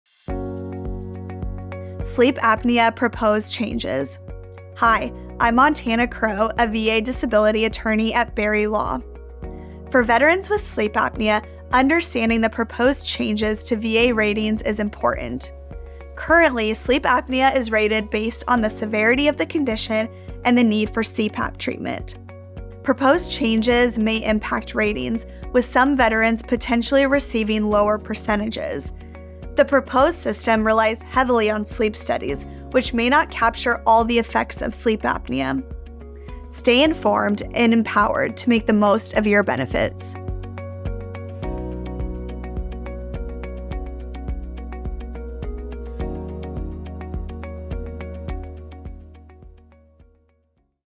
VA Proposes Changes to Sleep Apnea Disability Ratings
The VA has announced proposed changes to how it rates service-connected sleep apnea for disability compensation. Currently, a 50% rating is common for Veterans prescribed a CPAP machine. Under the new rules, ratings would be based on the effectiveness of treatment as measured by sleep studies.

Asymptomatic sleep apnea would receive 0%, while 10% would apply if treatment provides incomplete relief. 50% and 100% ratings would require treatment to be ineffective or impossible due to other conditions. However, basing ratings solely on sleep studies fails to account for daytime symptoms like fatigue and cognitive issues that impact Veterans' ability to work.

Veterans with sleep apnea should file service connection claims immediately to be rated under the current, more favorable criteria. The public can also submit comments on the proposed rule change until April 18. If you need help with a sleep apnea claim or appeal, contact an experienced Veterans disability attorney.

BERRY LAW - AMERICA'S VETERANS LAW FIRM